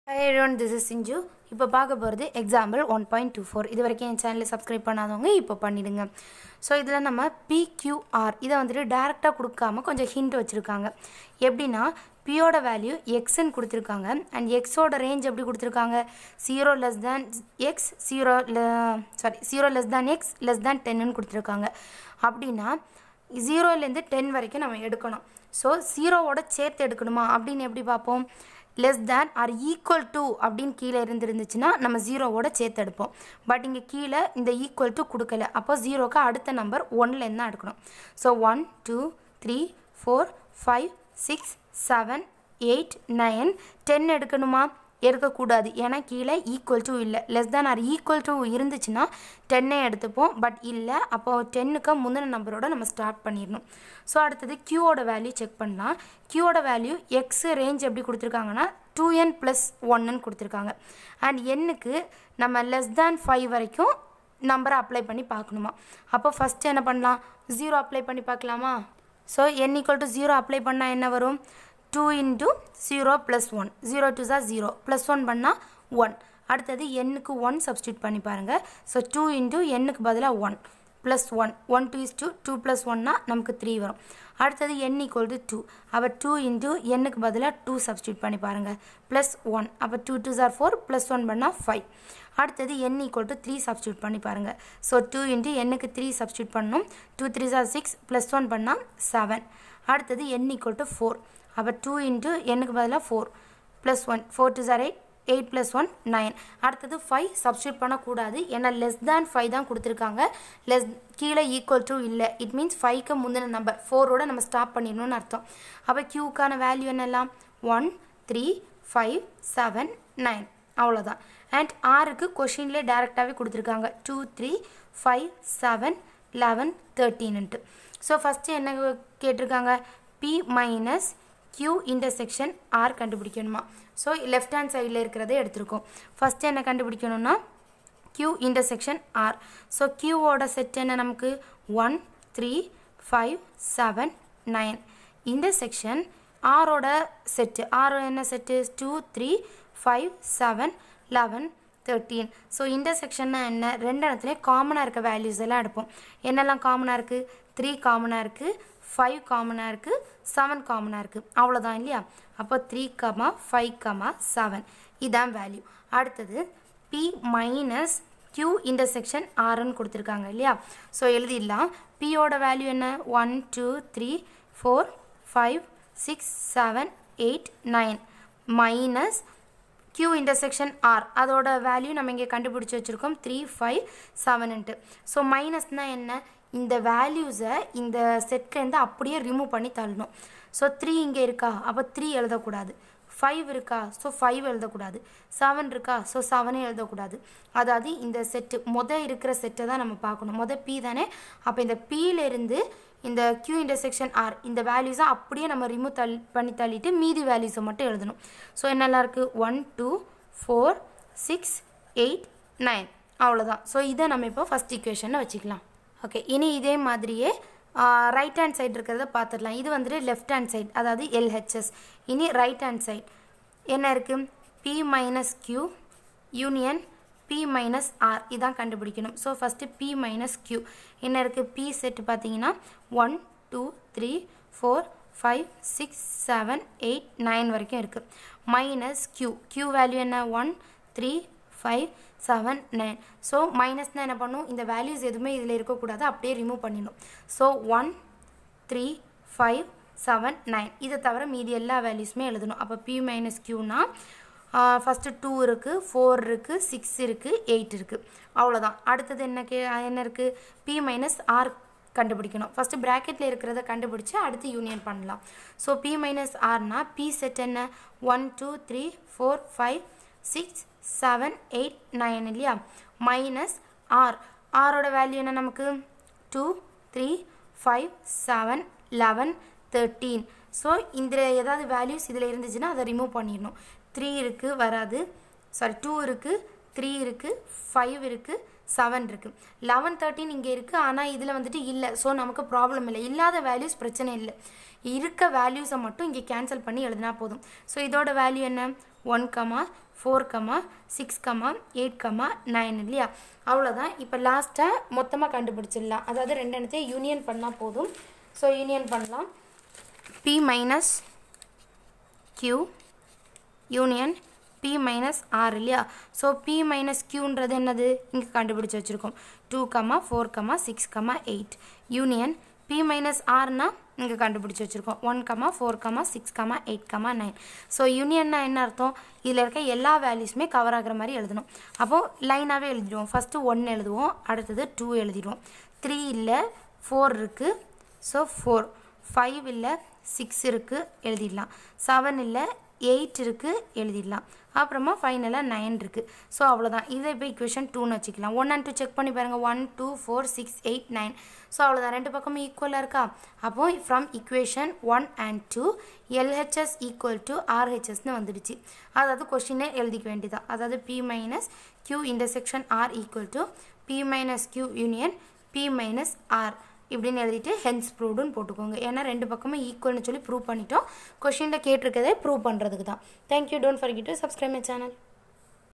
9th Maths Chapter 1 Example 1.24 Samacheer State board Tamil English medium
Hello Everyone This is Sinju. Here I explained Example 1.24 from Chapter 1 for 9th std. Also I Explained the problem from very basic. Watch the video and then try to solve the problem on your own. It will surely help. All the Best to score full marks in maths
It is a syllabus for State board/matric/Samacheer/English medium which is explained in tamil